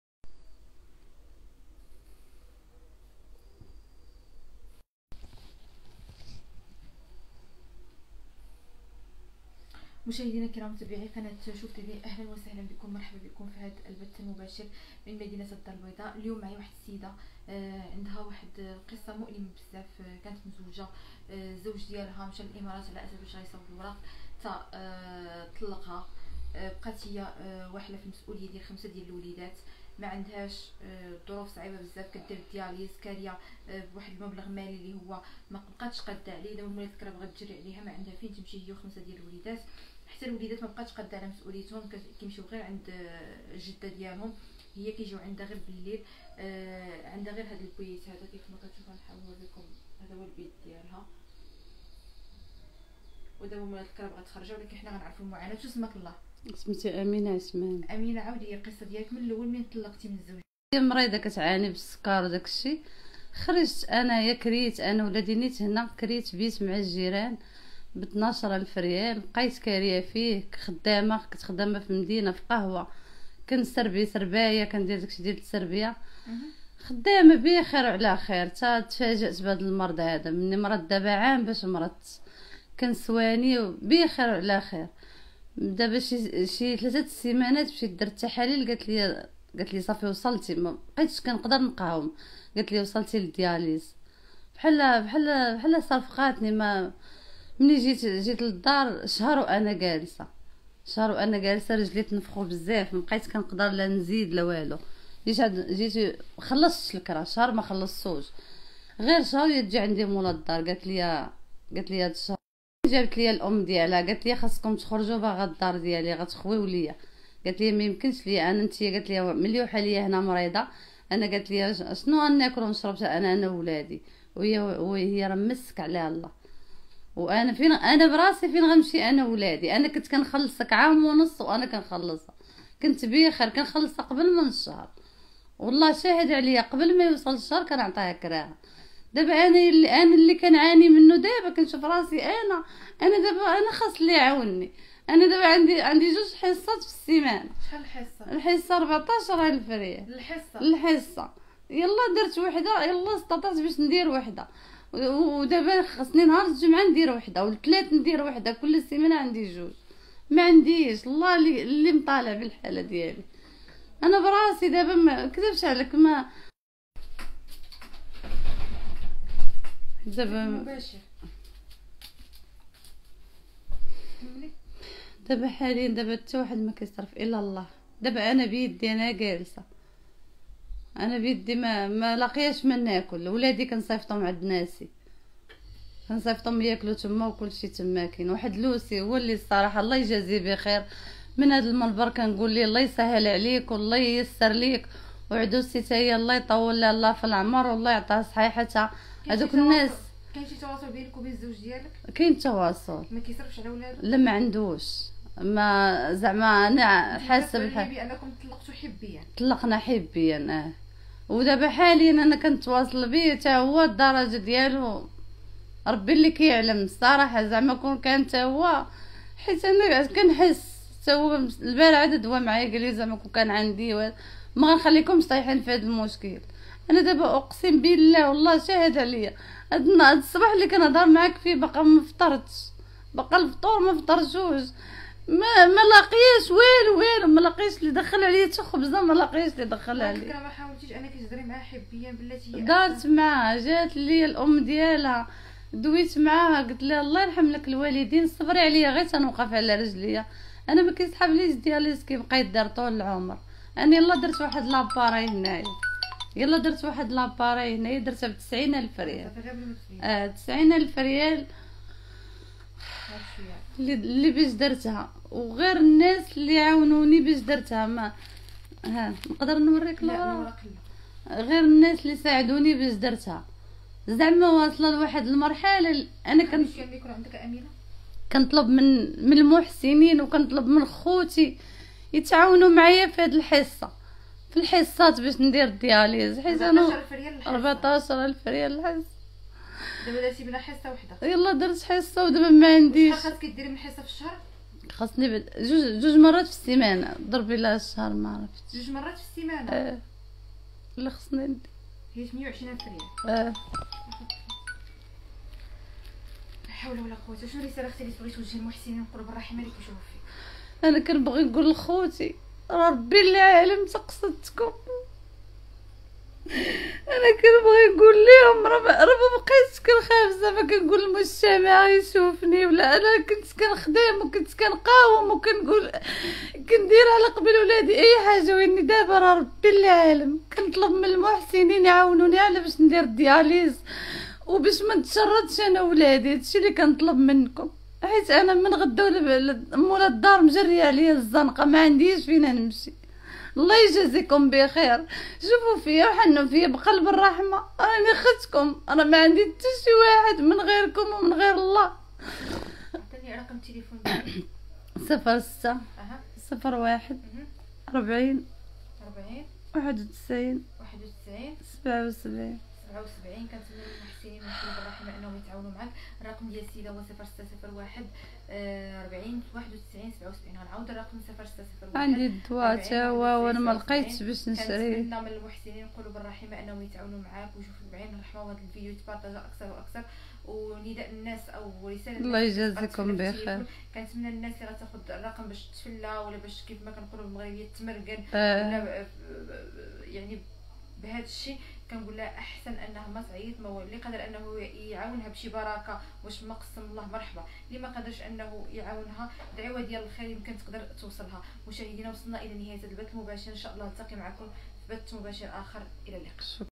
زوجة طلقها راجلها ومشا للإمارات ومرضات بالدياليز وسمح ليها فـ5 الوليدات ومول الكراء بغا يجري عليها
مباشرة من الدار البيضاء:دموع زوجة طلقها راجلها ومشا للإمارات ومرضات بالدياليز وسمح ليها فـ5 الوليدات ودابا مول الكراء بغا يجري عليها للزنقة هي وولادها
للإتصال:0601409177